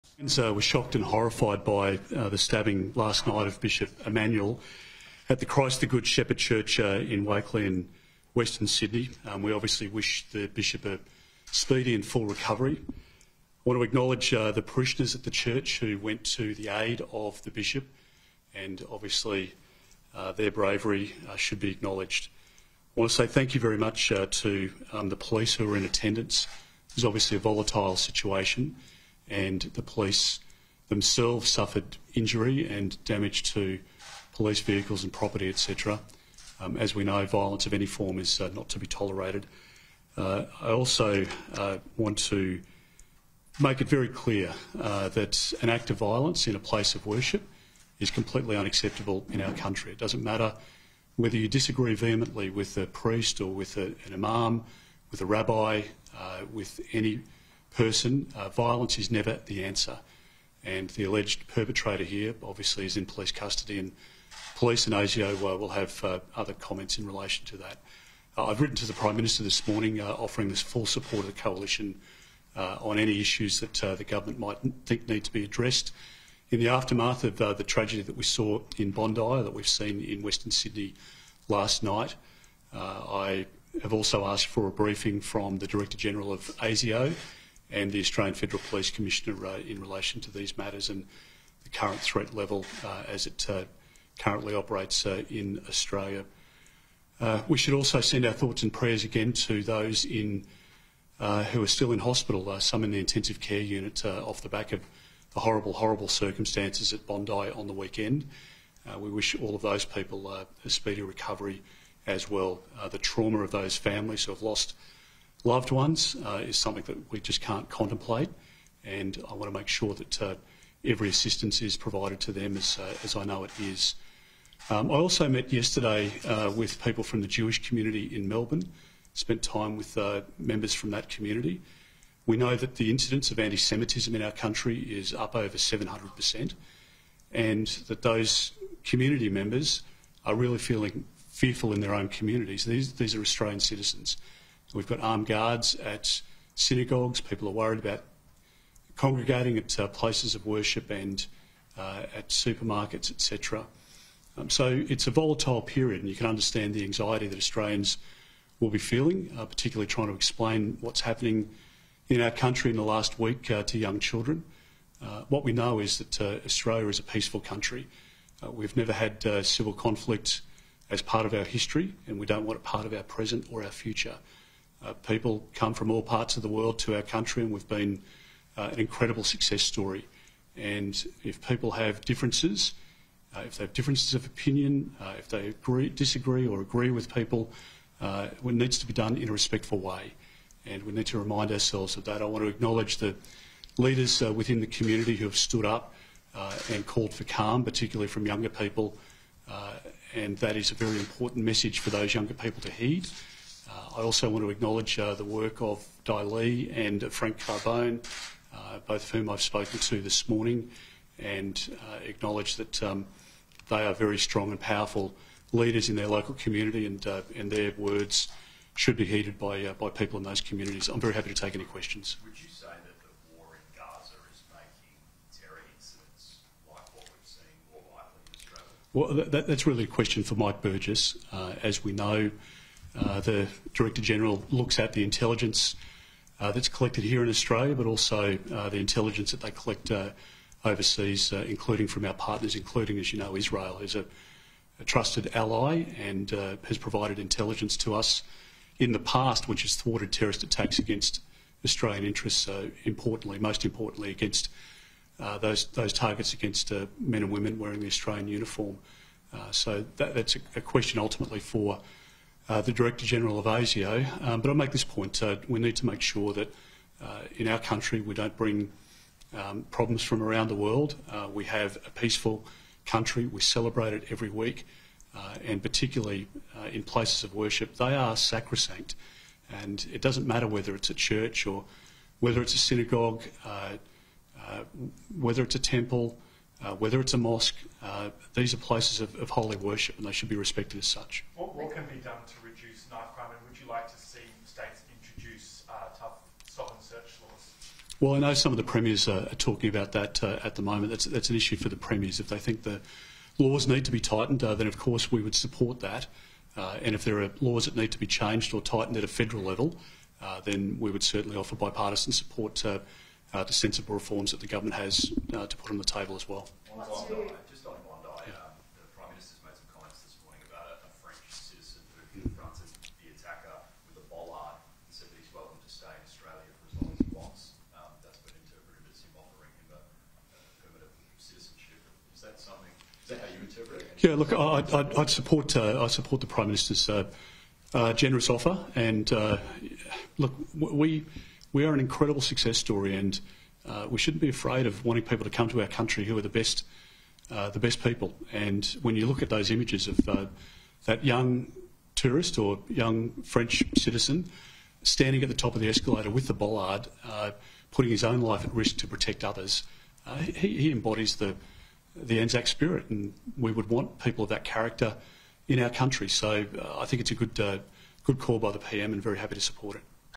‘Bollard man’ embodies Anzac spirit: Peter Dutton supports PM's offer of citizenship
Opposition Leader Peter Dutton says the French citizen who tried to stop the Bondi Junction attacker during his stabbing rampage “embodies the Anzac spirit,” and says he supports the PM’s offer of citizenship.

Prime Minister Albanese granted “bollard man” Damien Guerot, who confronted the Bondi Junction attacker, the ability to stay in Australia for as long as he likes, even going as far as enabling him to become a citizen.

“I support the Prime Minister’s generous offer,” Mr Dutton told media on Tuesday.

“We [Australia] are an incredible success story, and we shouldn’t be afraid of people wanting to come to our country who are the best people.

“When you look at those images of that young tourist, or young French citizen… putting his own life at risk to protect others, he embodies the Anzac spirit.

“We would want people of that character in our country so I think it’s a good call by the PM and I’m very happy to support it.”